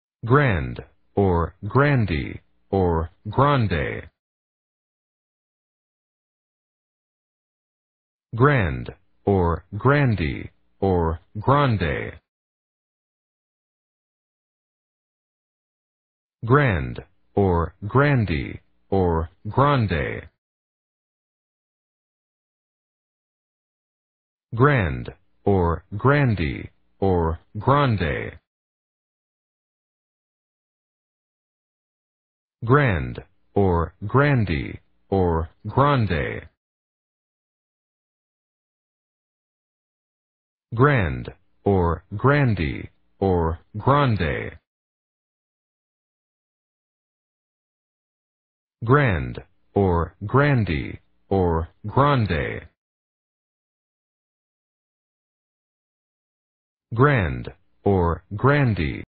How to pronounce Grande | English pronunciation
Learn how to pronounce Grande in English
GRANDE
Pronunciation of Grande: /ɡrænd/